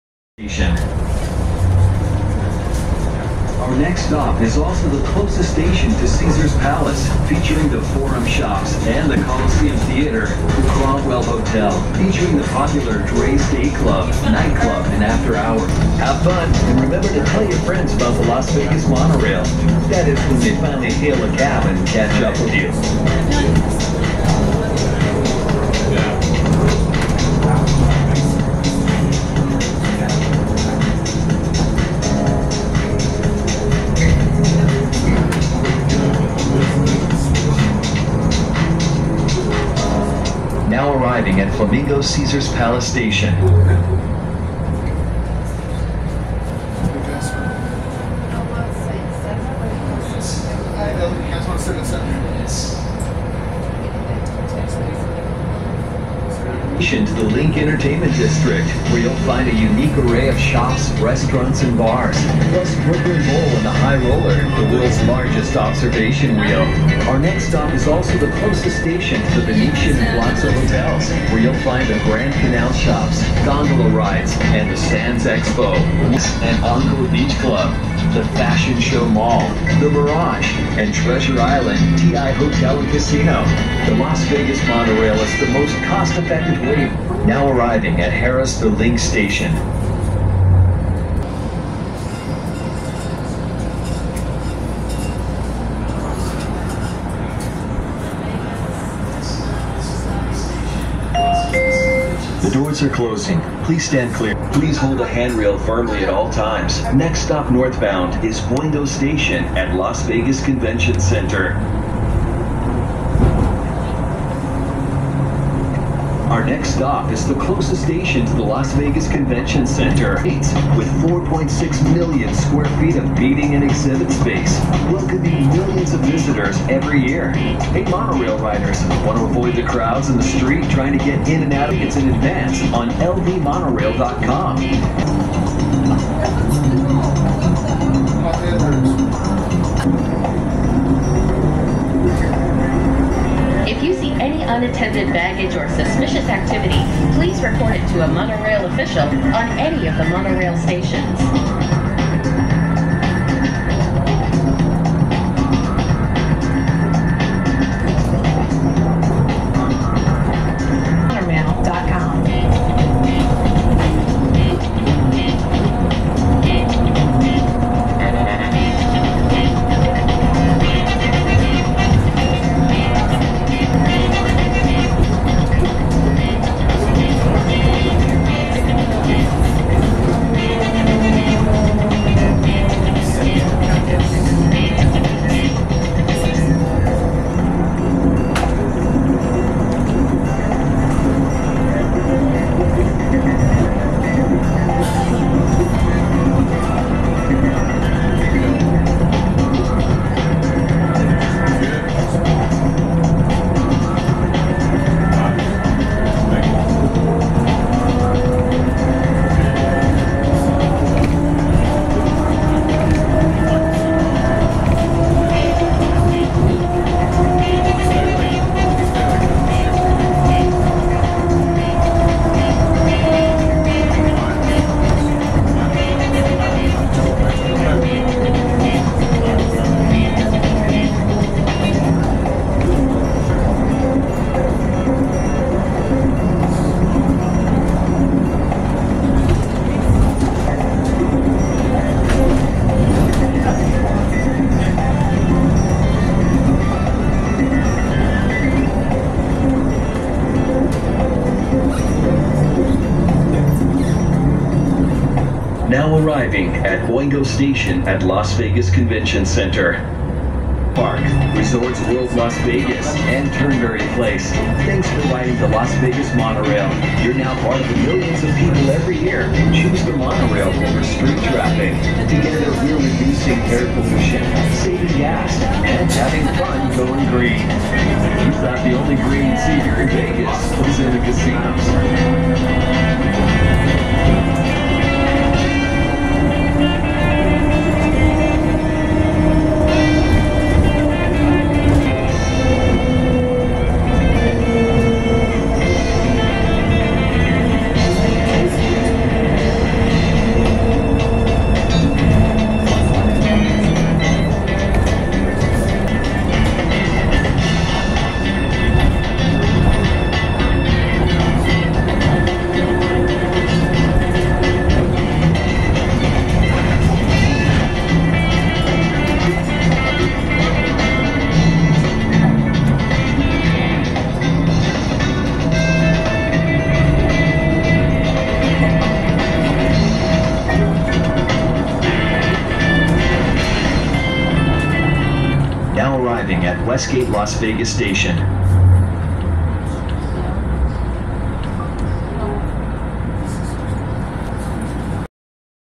Las Vegas Monorail
Riding the monorail at Las vegas was convenient. I liked the views and the air conditioning in the monorail was perfect for cooling down between travels.